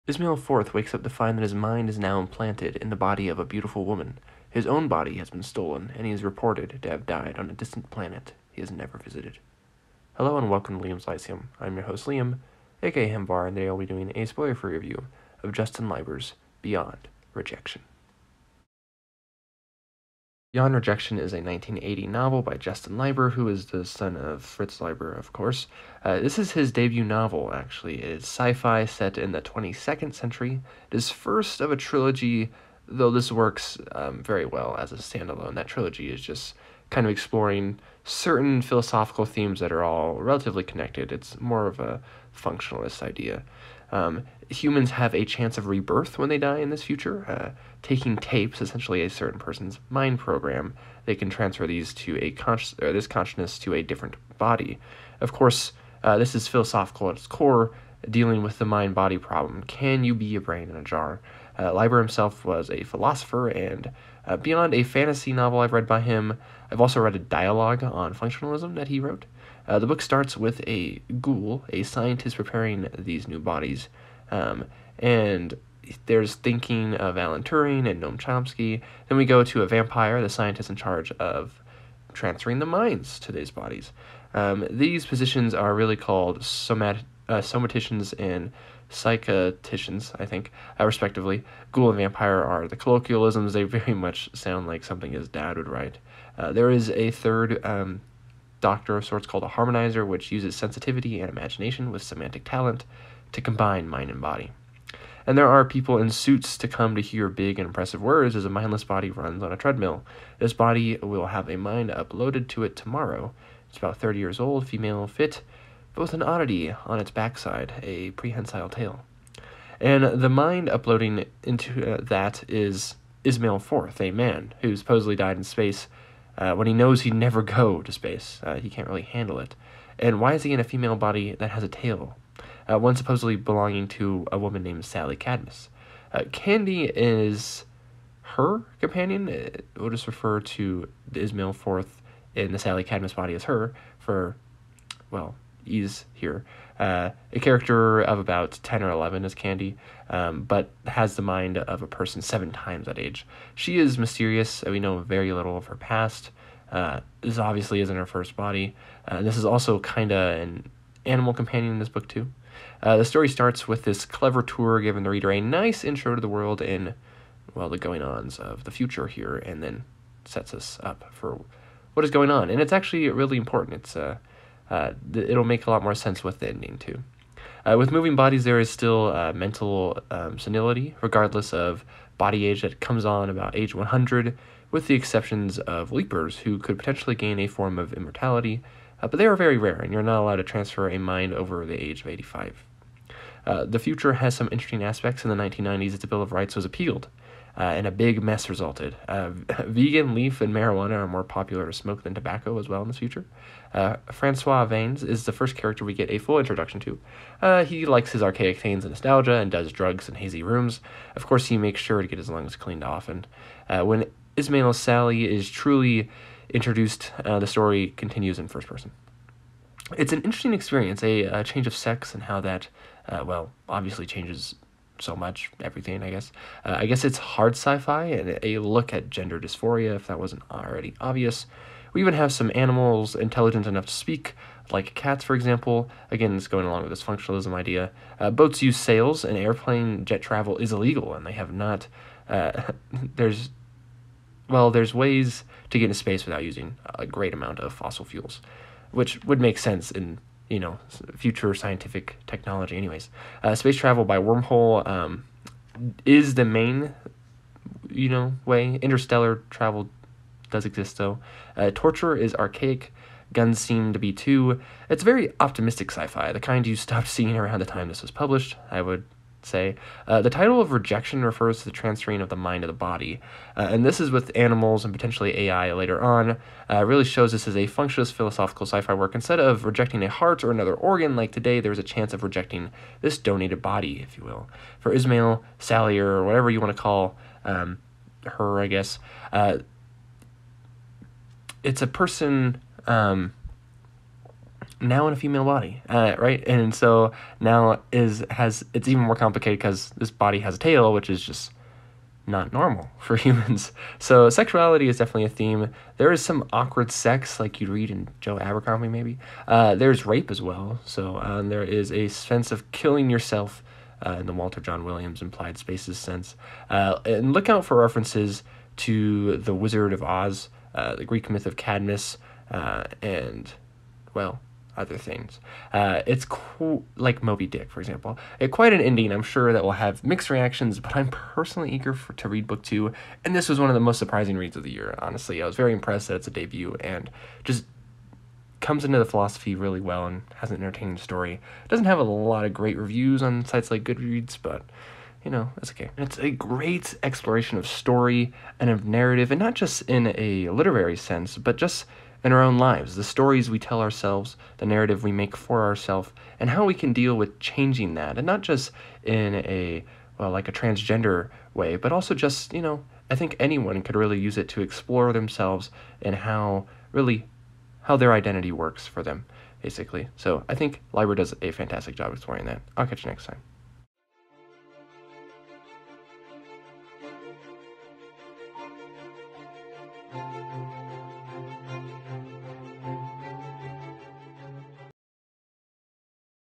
Beyond Rejection by Justin Leiber | Spoiler Free Review
A great philosophical exploration of the future and identity and story.